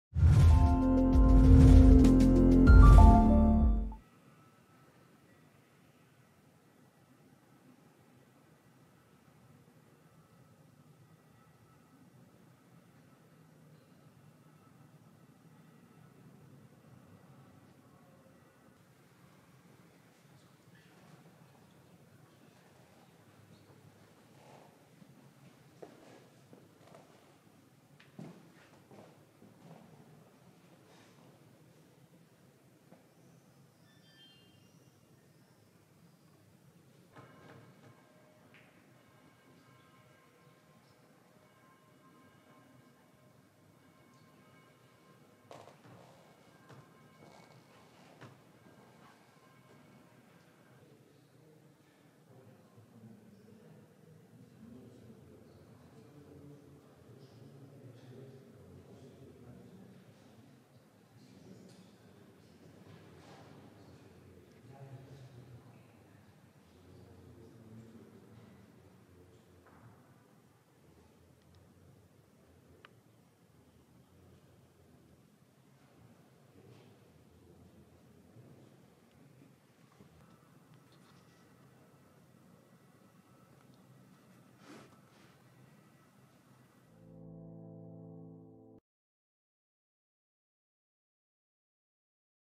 El interior de la Vera Cruz muestra el desastre
Imágenes cedidas por la Archidiócesis de Valladolid.